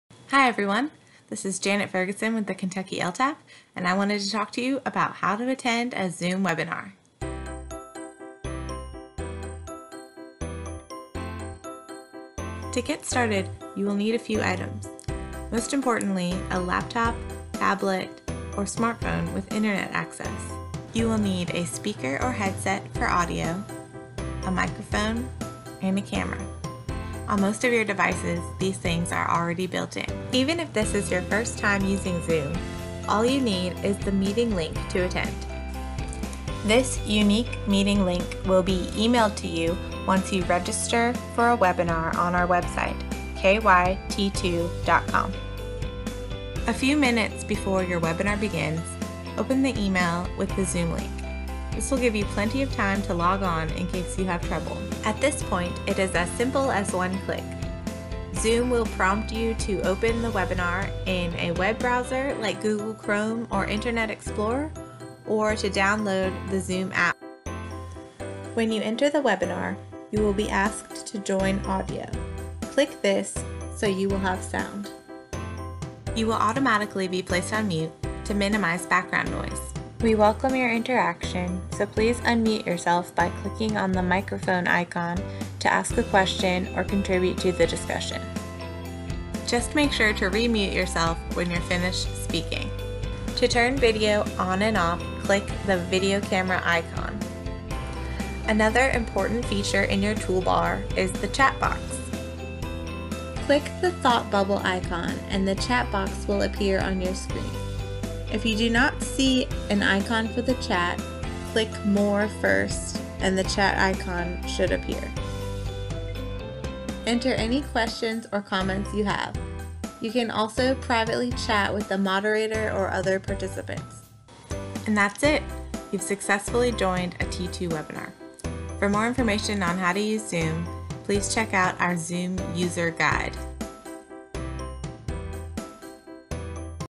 KTC T2 Zoom Tutorial
A brief overview on how to use Zoom for our online training courses and webinars.